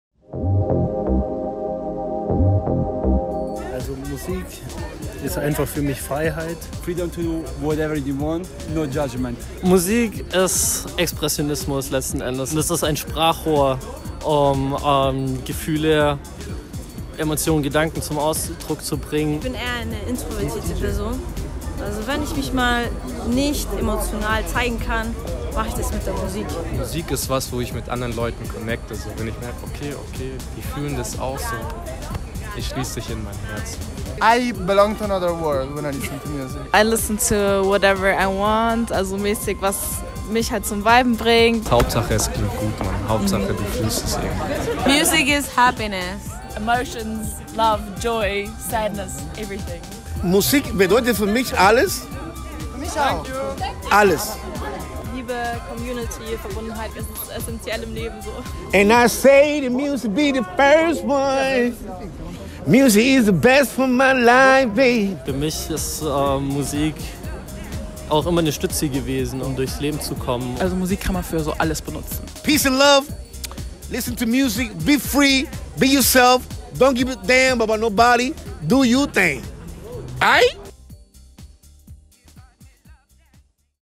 COZY TALKS 002 - Music is..
Was bedeutet Musik für dich?

Wir haben euch gefragt!